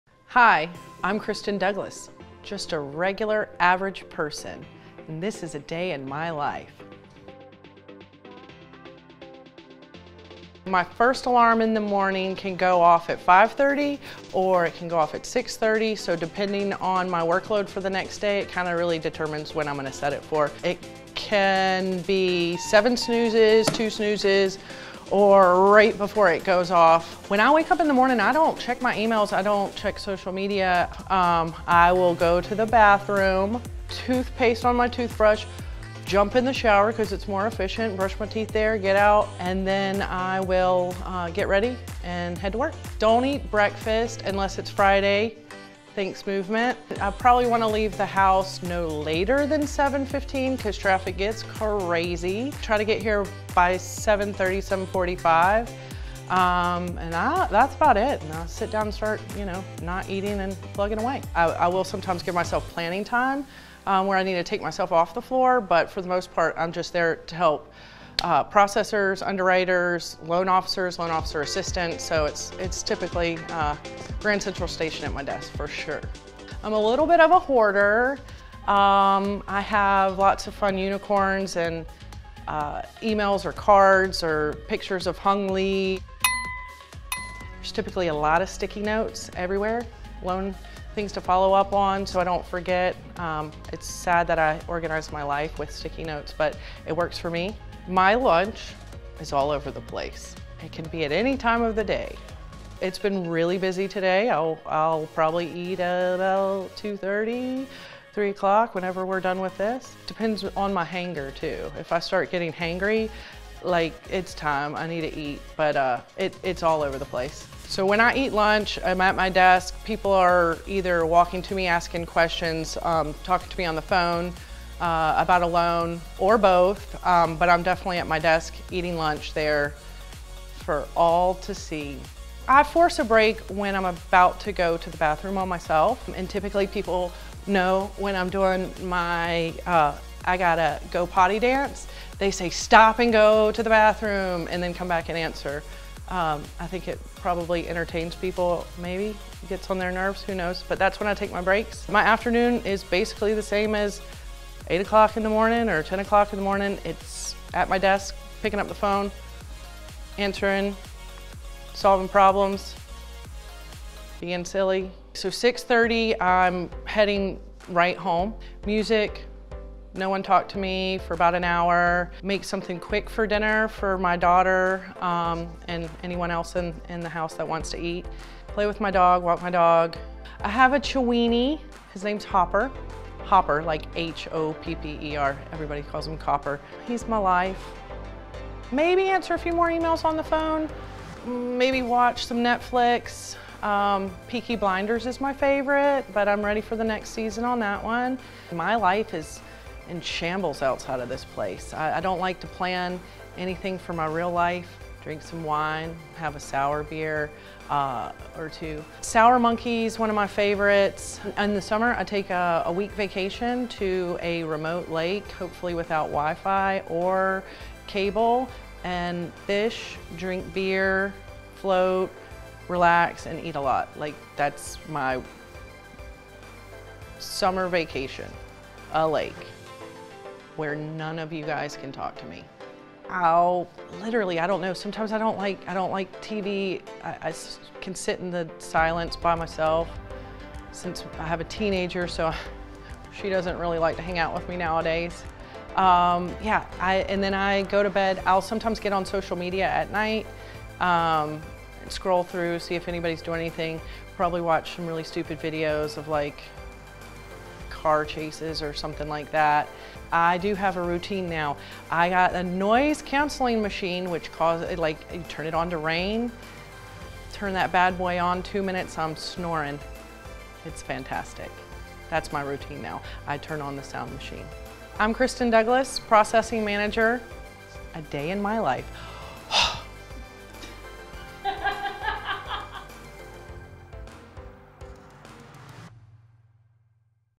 Day in the life | Christin Douglas
Ever wonder what it's like to be a processing manager? For Christin Douglas it involves answering questions and fixing problems, while trying to fit in bathroom breaks every once in a while. Check out this edition of Day in the life!